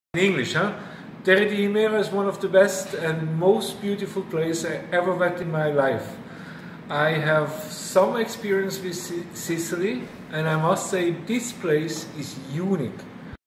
terre di himera charming sicily agritrurismo
agriturismo di charme near Cefalu and Palermo in the Himera  archaelogical park .
real sicilian countryside country kitchen local and organic food